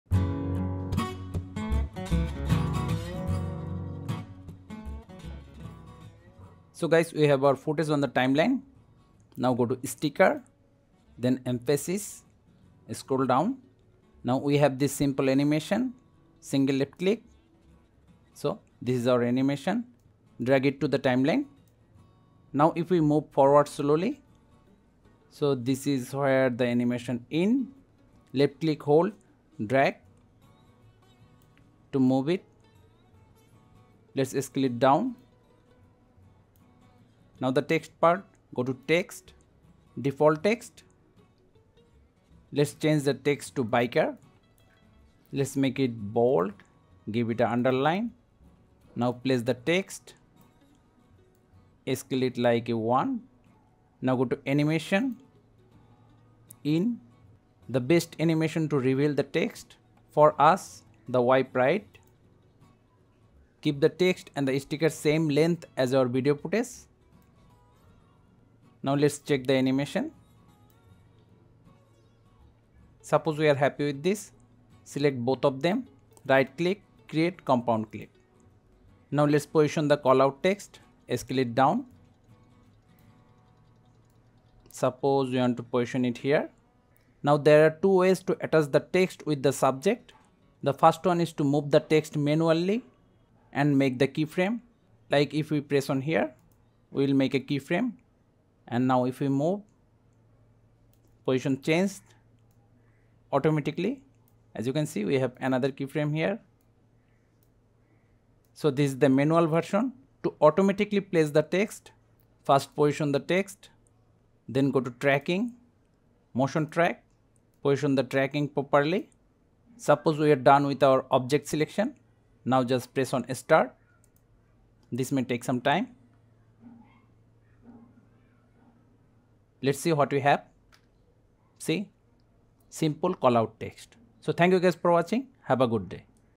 Call Out text Animation with Tracking | CapCut PC Tutorial
How to create call out text and attach the text with the subject or object in CapCut by using motion tracking. This is a beginner friendly tutorial on CapCut video editing for Windows and Mac OS users.